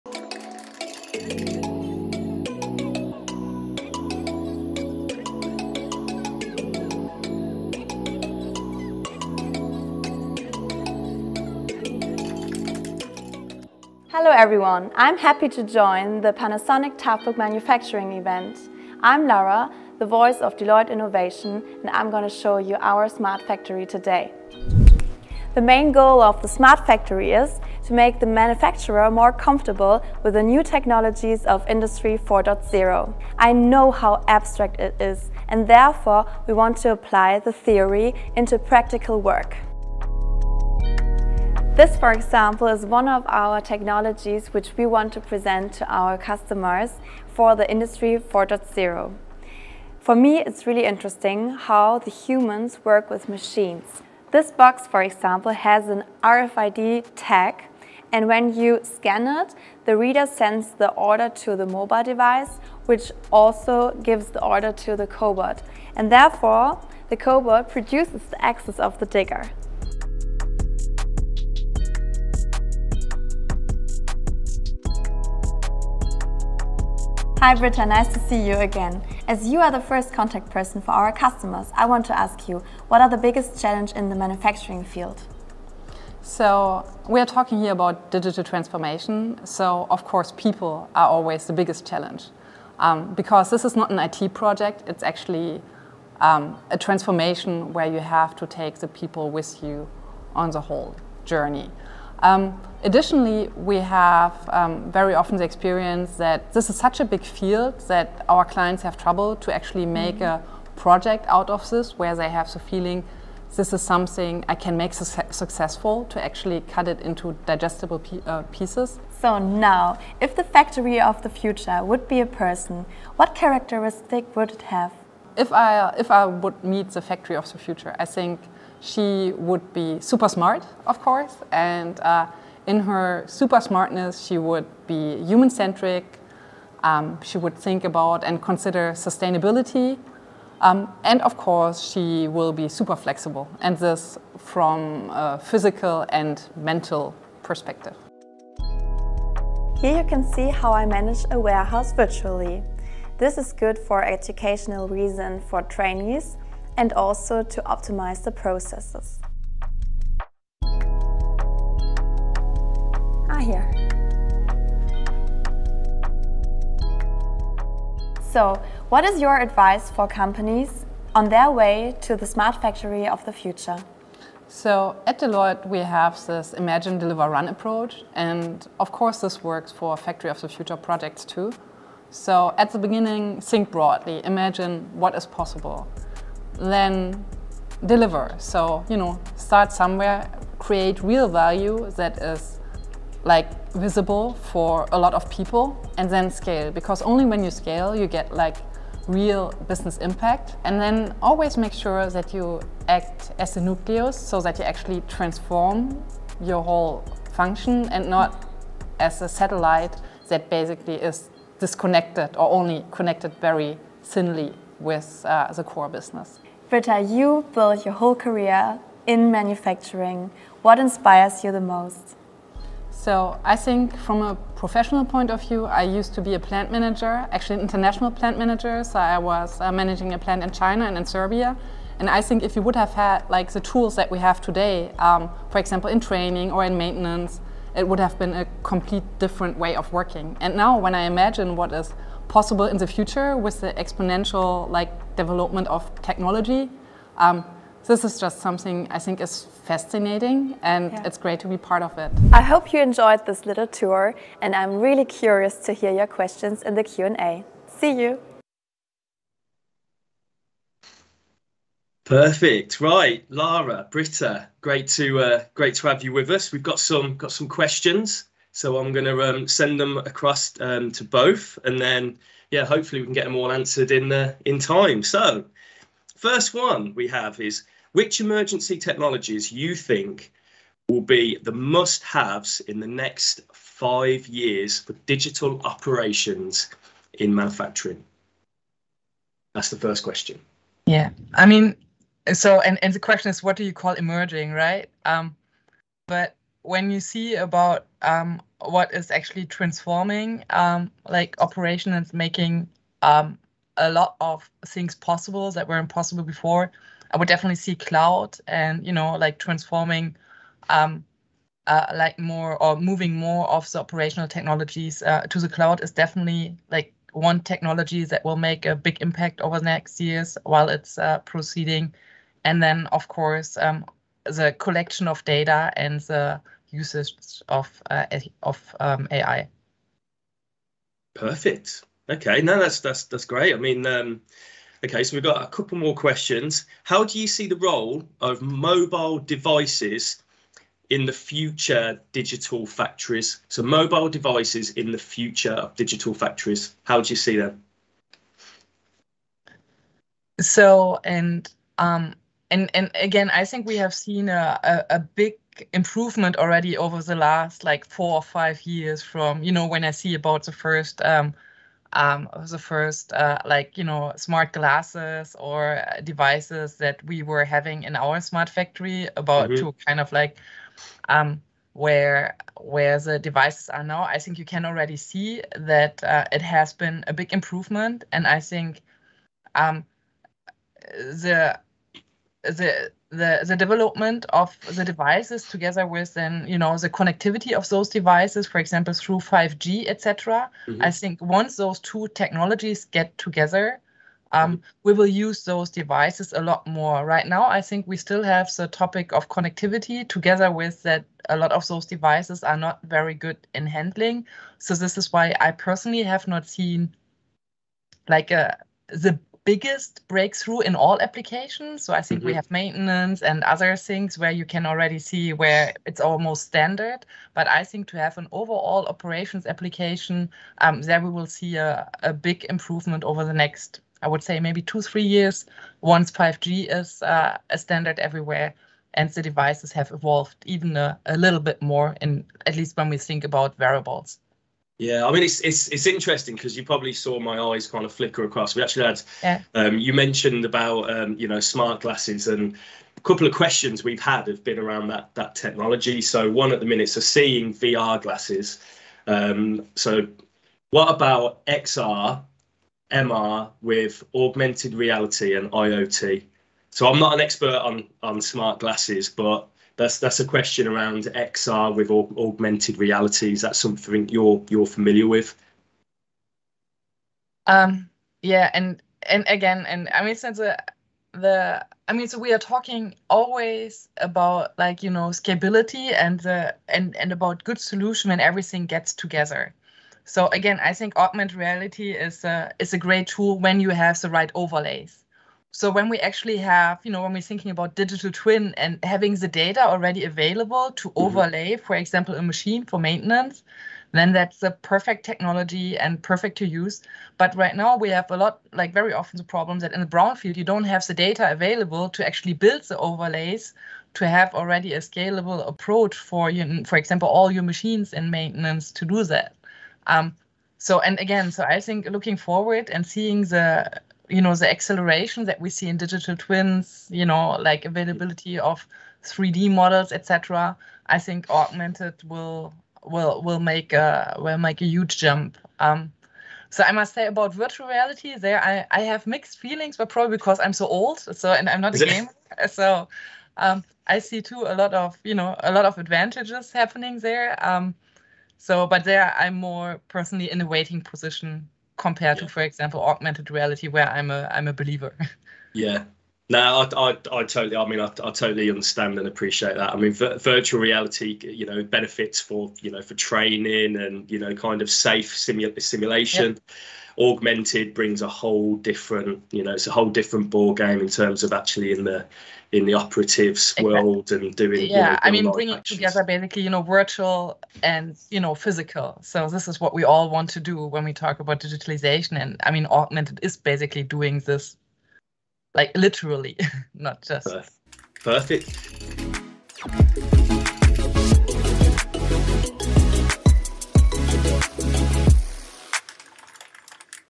Deloitte’s Smart Factory of the future tour
Deloitte Innovation representative Lara Bothur showcases the factory of the future, followed by an overview of smart manufacturing from Factory Director Britta Mittlefehldt.